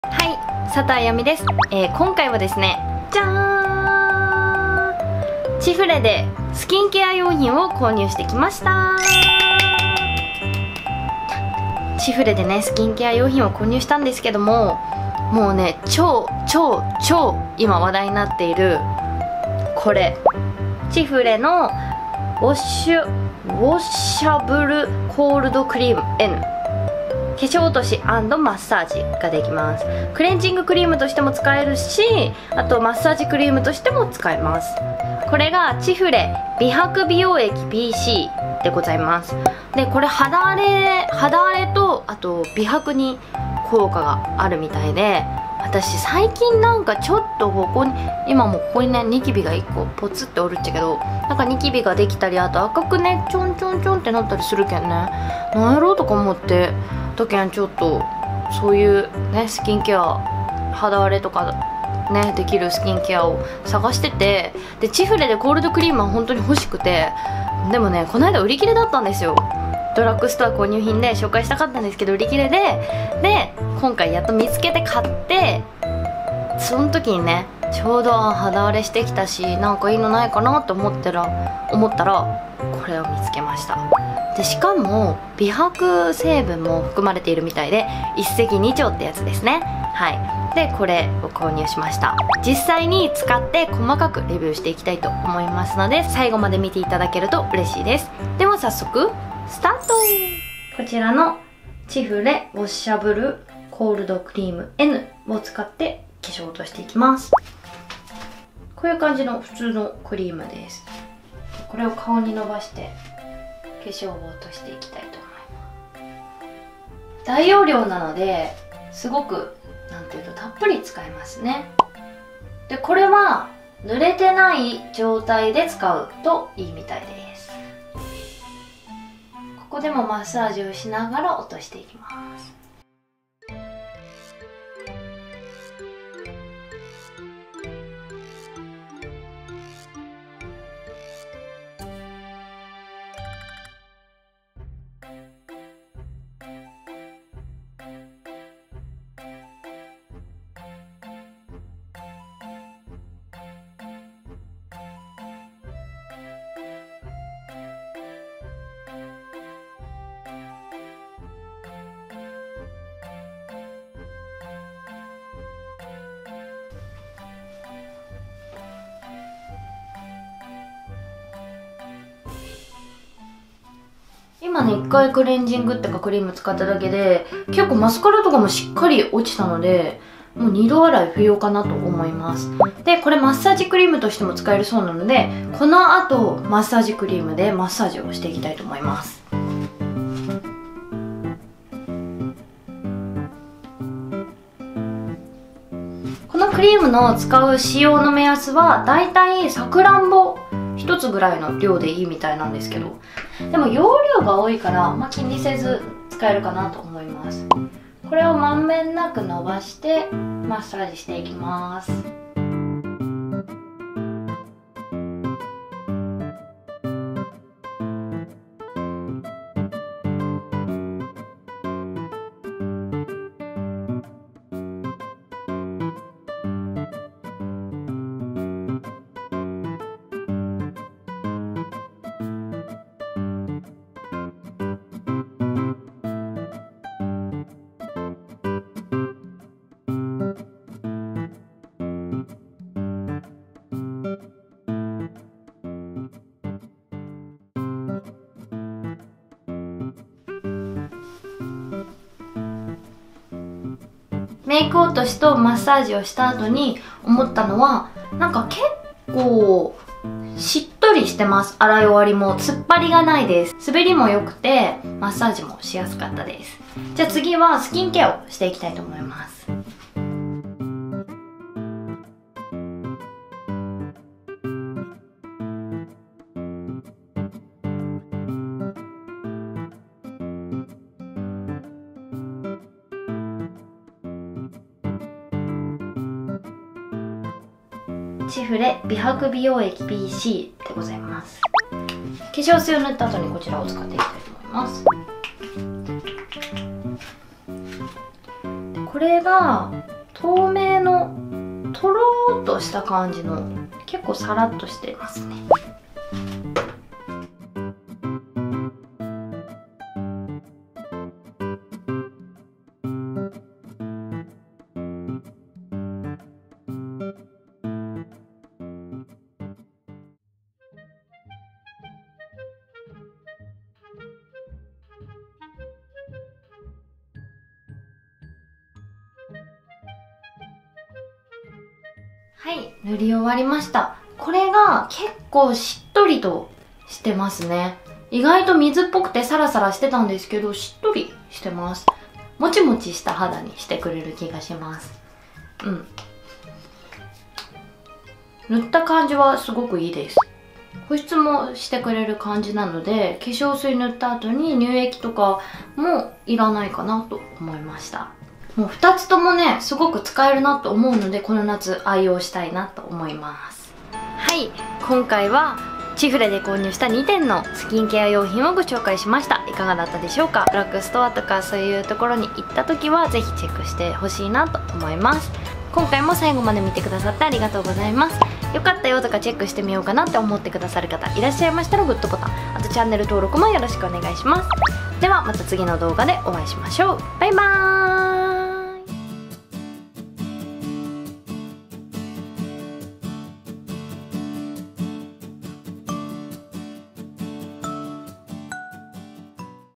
【話題の】ちふれで超人気プチプラスキンケア商品2点購入！実際に使ってレビューしてく！【コールドクリーム 美容液】
はい！佐藤あやみです！
今回は！！！

売り切れ続出でやっとやっと手に入れました！ちふれの話題のアレ！

と、最近肌の調子が良くないので美容液も購入してみました！

ふたつともこんなに優秀なのに激安！！！！びっくりです。。やっぱちふれすごいわぁ

今回購入した商品はこちら！
ちふれ ウォッシャブルコールドクリームN （クレンジング・マッサージ）
ちふれ 美白美容液VC

今回も最後まで見てくださってありがとうございます！！

【音源】
○DOVA-SYNDROME

○ファンレター等の宛先はこちらです！！
〒106-6137
UUUM株式会社 佐藤あやみ宛